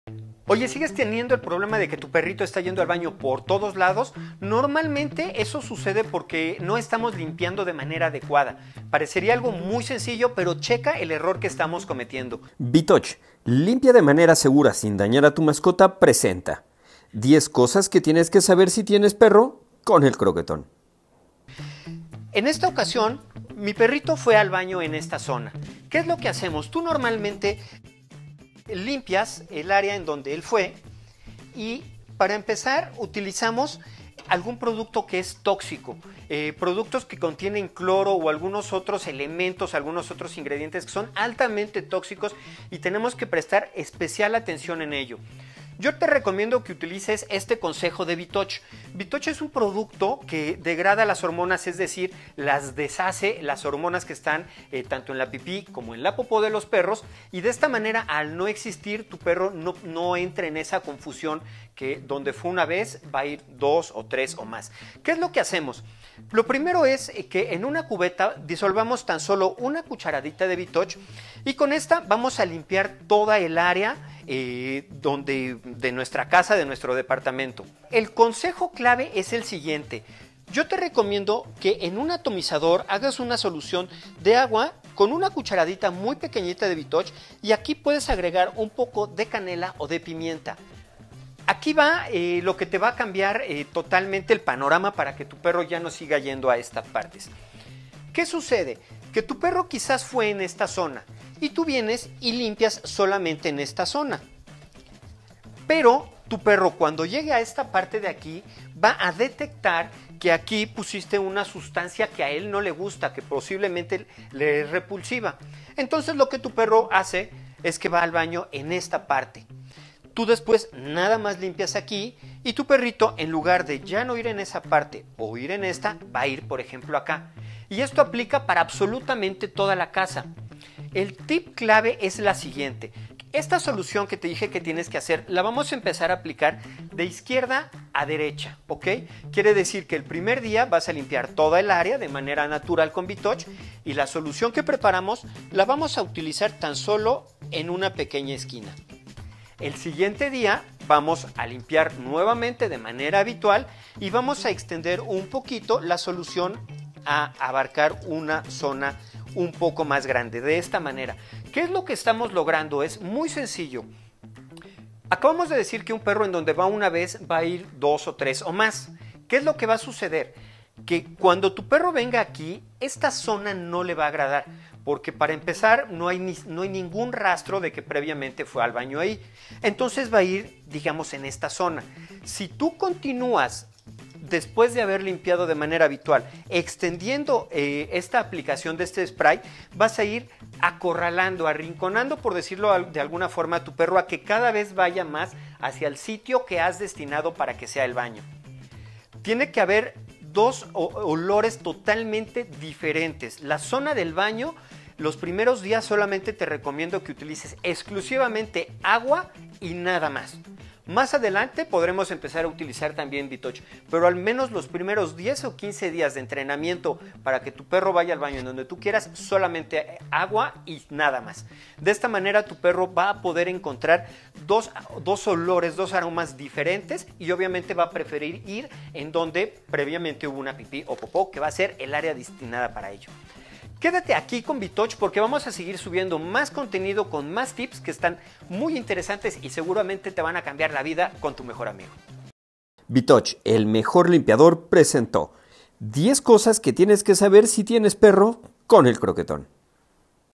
ELIMINA OLORES, BITOCH PRODUCTO PARA LIMPIAR PIPI Y POPO DE PERRO, CONSEJO PARA QUE NO HUELA MAL
00:00 errores al limpiar donde mi perro va al baño
00:51 Bitoch limpiador de mascotas
01:25 consejo para que no vaya al baño
02:22 tip clave para quitar malos olores

Esta es la forma de limpiar de manera adecuada el lugar donde tu perro hace del baño y asi mantener limpio y sin olor.
El mejor limpiador para areas de mascotas es Bitoch.